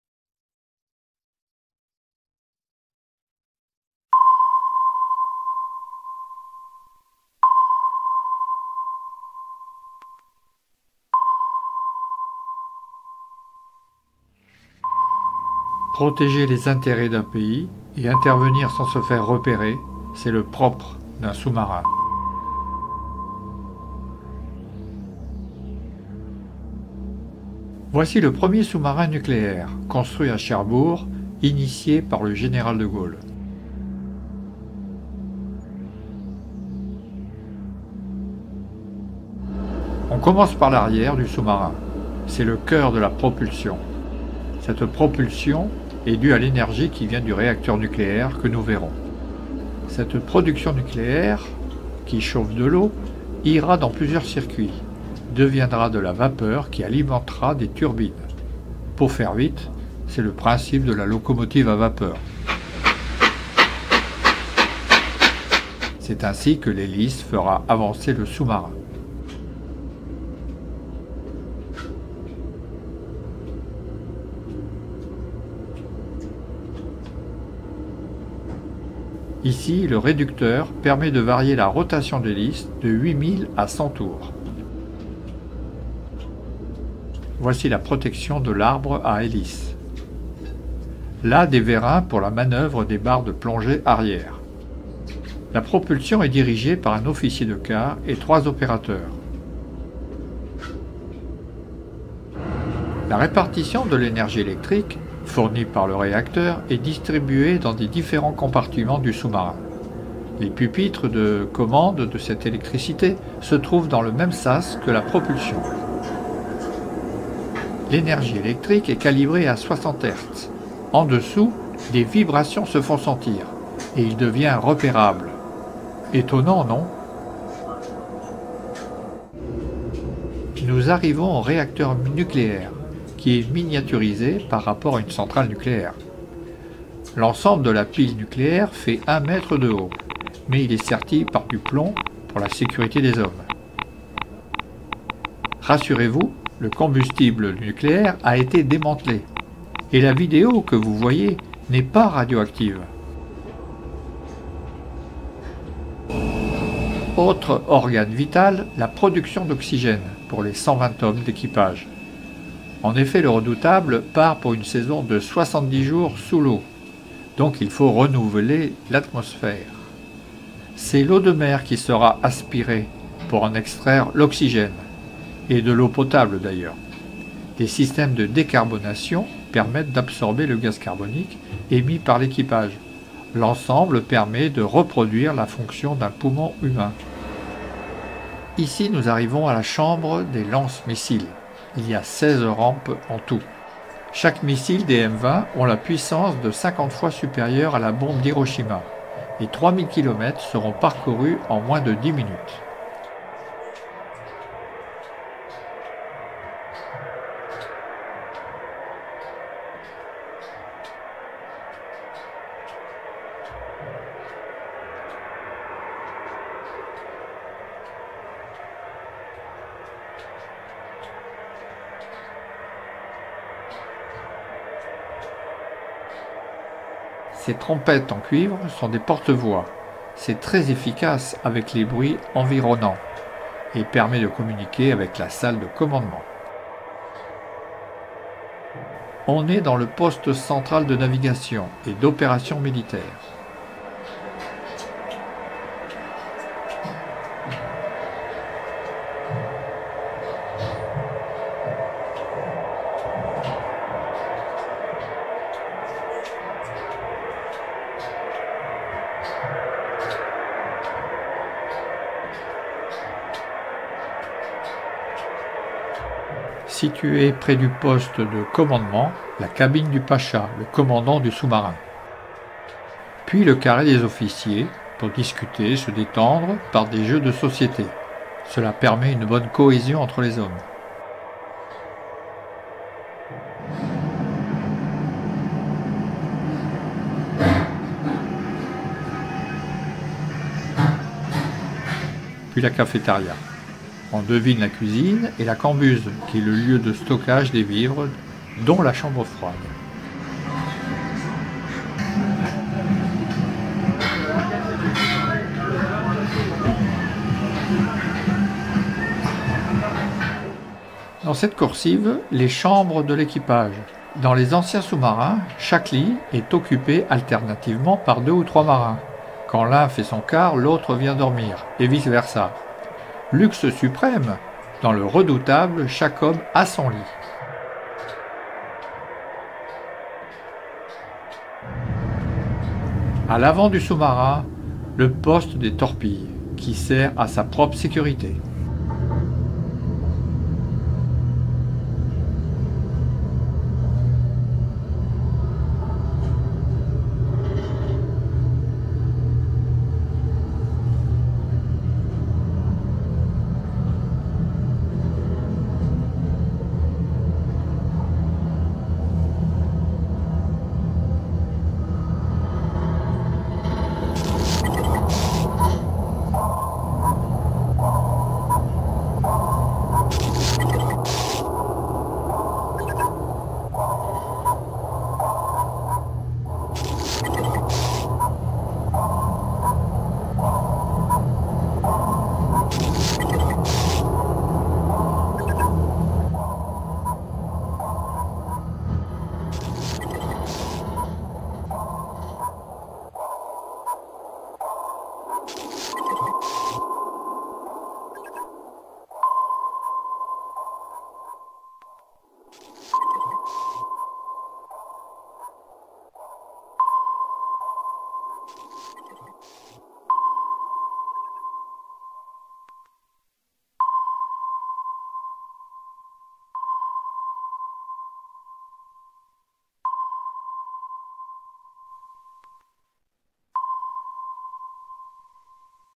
Sous-marin nucléaire - Le Redoutable
Voici le 1er sous-marin nucléaire, construit à Cherbourg, initié par le Général De Gaulle.
SNLE = Sous-marin Nucléaire Lanceur d’Engins !
La visite intégrale du Redoutable, de 120 mètres de long.
🔻 Plus d'infos 🔻 sous-titres 🔻

♥ Visite des différents compartiments du sous-marin nucléaire "Le Redoutable" long de 120 mètres.
- 00:40 La salle des Machines
- 01:43 L'usine d'électricité
- 02:10 Le réacteur nucléaire
- 02:37 La production d'oxygène
- 03:05 La chambre des 16 lance-missiles
- 03:56 Le poste de commandement
- 04:33 Le carré des officiers
- 04:44 Le restaurant, la cafétéria
- 05:11 Les chambres de l'équipage
- 05:31 Le poste des torpilles
C'est parti pour un plongeon de 70 jours, sans voir le ciel !

Bruitages originaux
👉(click on the bell!) 👈 to be informed about new videos